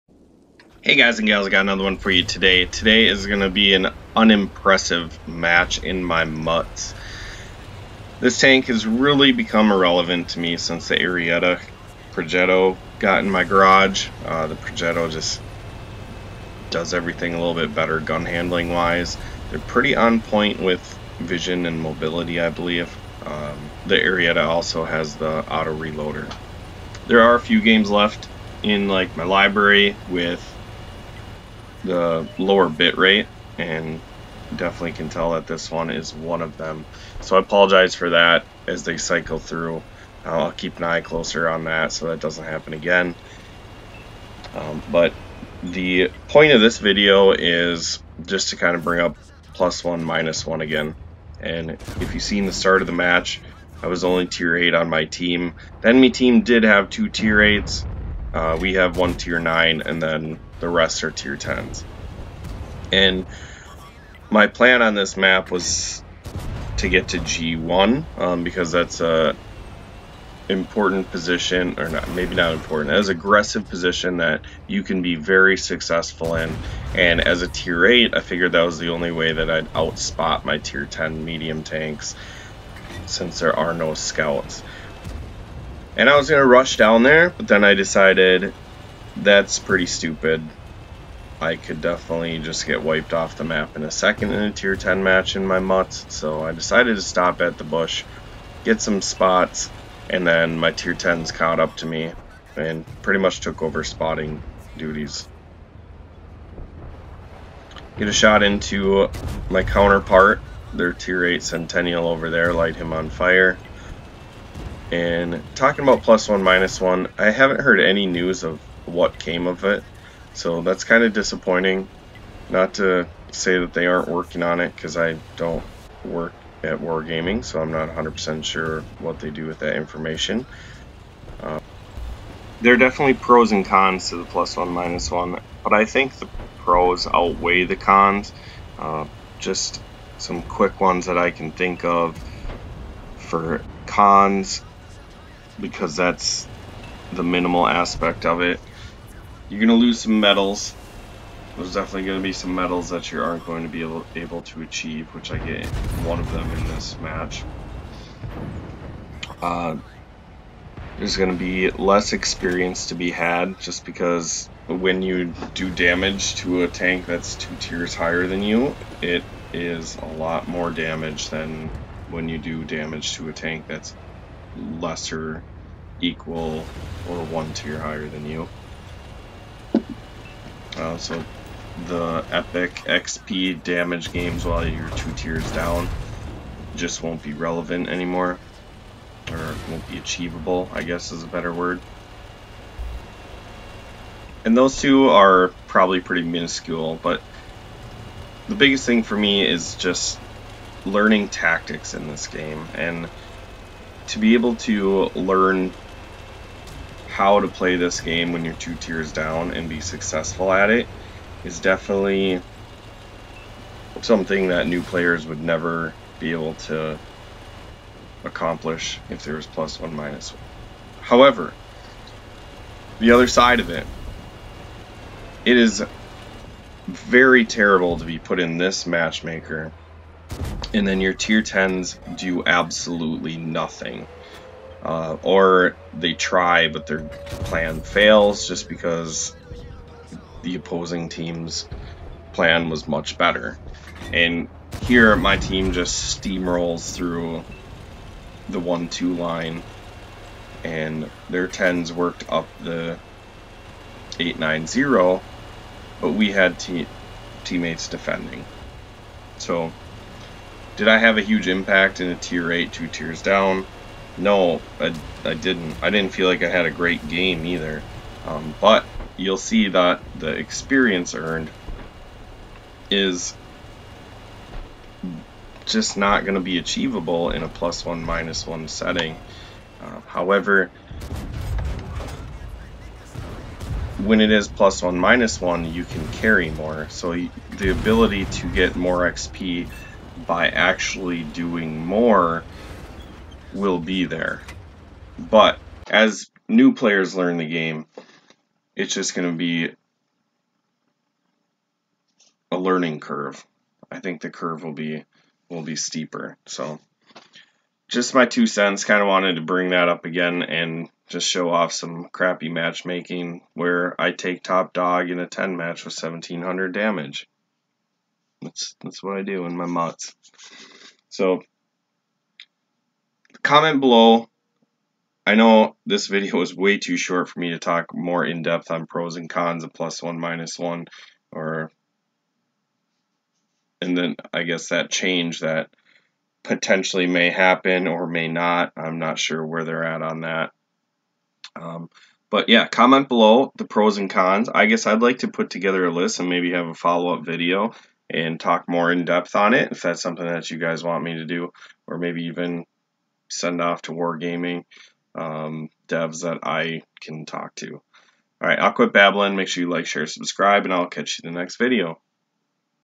Matchmaker Changes?
Stuck in a ten match in my Mutz, an unispiring game, however still end top Dawg!  Dabble into +1/-1 matchmaker talk and how these games would be unacheivable in that change.